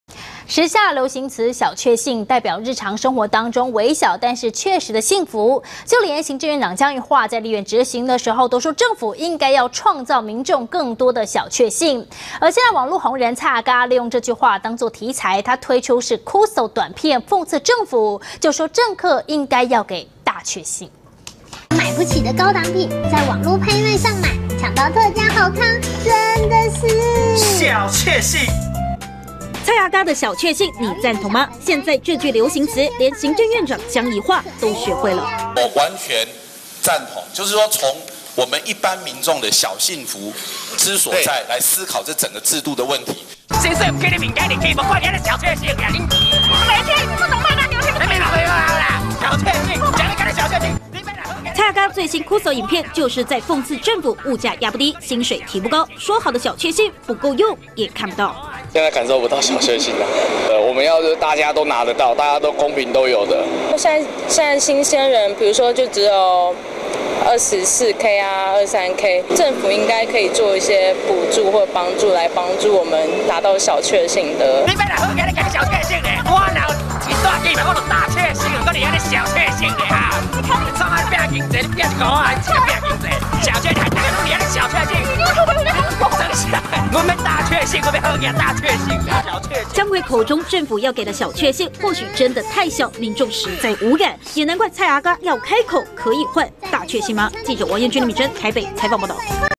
[中天新聞] 蔡阿嘎批物價漲薪水低KUSO影片要「大確幸」 (2014.03.09)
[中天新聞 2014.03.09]
時下流行詞「小確幸」，代表日常生活中，微小但確實的幸福，就連行政院長江宜樺在，立院質詢時也說，政府應該「創造民眾更多小確幸」，現在網路紅人蔡阿嘎，也用這句話當作題材，推出最新KUSO短片，諷刺政府薪水沒漲卻讓物價飆漲，要政客們給「大確幸」。